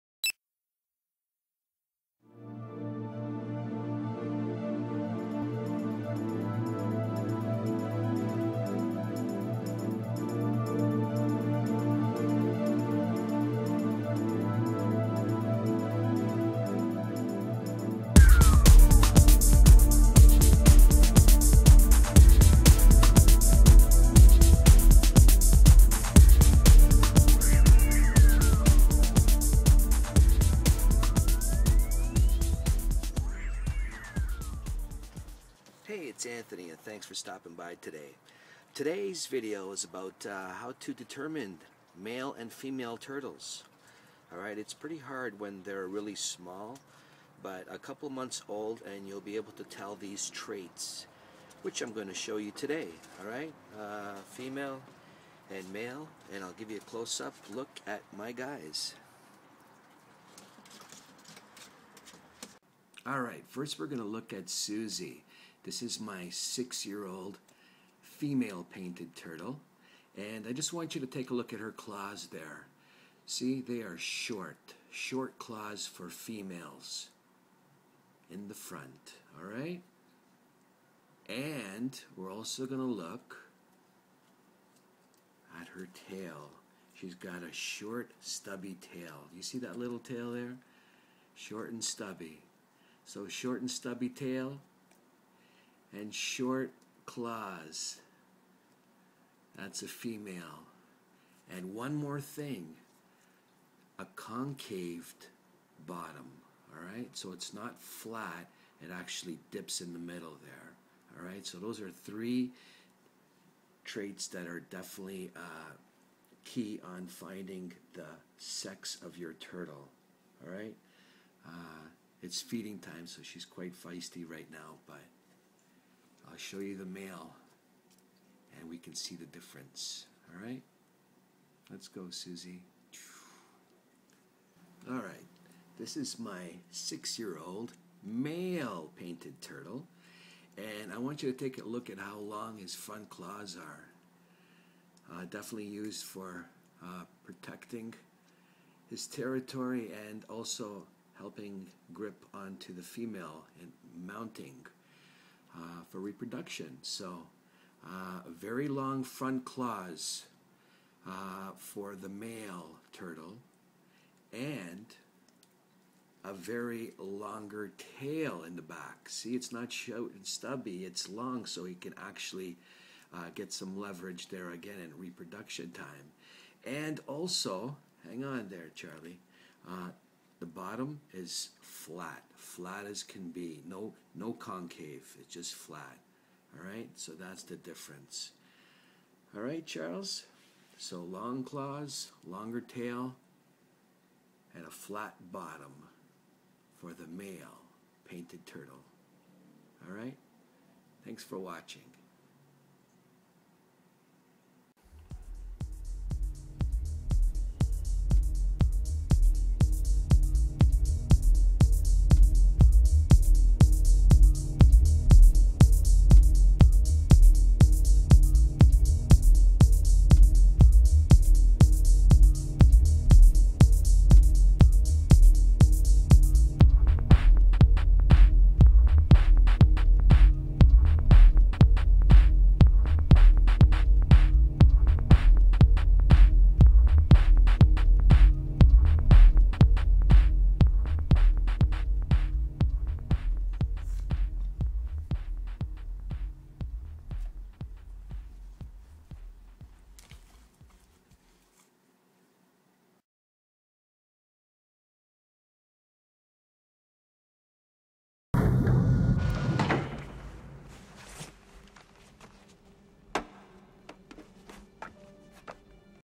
How To Tell if Your Red Eared Slider is Male or Female
(Official TURTLE-MAN Video) In this fun and educational video I will show you with detail how to determine the sex of your pet or wild turtle. its easy and fun to watch...Is your turtle male or female - 3 different traits you should know - Turtle Facts you should need to know...Read More"